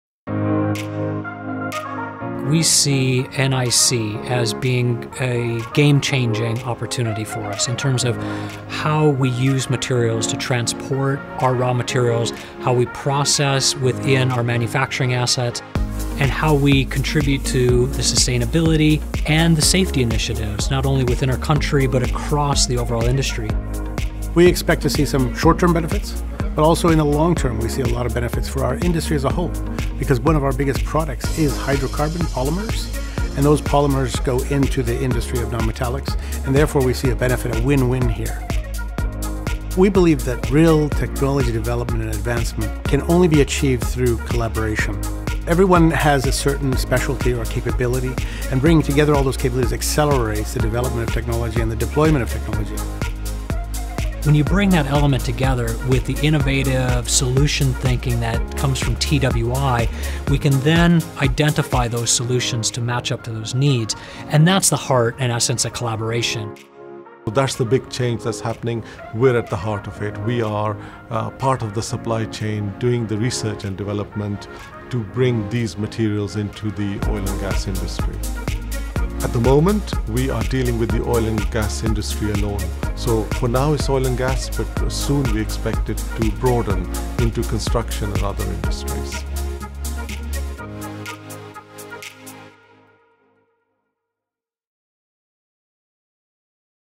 Interviews | Non-metallic Innovation Centre Inauguration Ceremony
On Monday 9th September 2019,  TWI Ltd (TWI), Saudi Aramco Technologies Company (AramcoTech) and the Abu Dhabi National Oil Company (ADNOC) officially opened the Non-metallic Innovation Centre (NIC), a research centre focused on innovation and advancing the use of non-metallic industrial applications.

During the launch ceremony, NIC interviewed Saudi Aramco VP and Chief Technology Officer Ahmad Al Khowaiter, ADNOC Chief Technology Officer Alan Nelson, and TWI Chief Executive Officer Aamir Khalid.